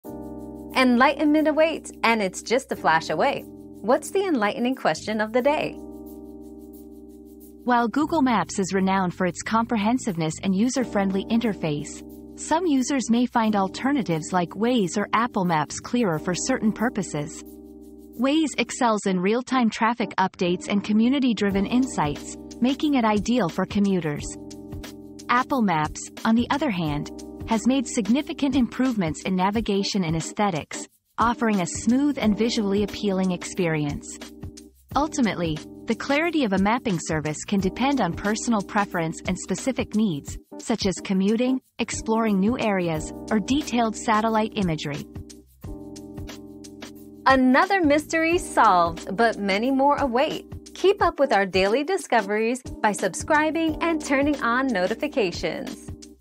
Is there anything clearer than Google Maps?
Navigating Clarity: Google Maps vs. Waze vs. Apple Maps 👉 Clarity Showdown 👉 Discover which mapping service offers the clearest navigation experience for your needs - Google Maps, Waze, or Apple Maps. From real-time traffic updates to smooth aesthetics, find out which platform suits you best.

Laura S. Harris (2024, March 8.) Is there anything clearer than Google Maps?
    🌐 AskAbout.video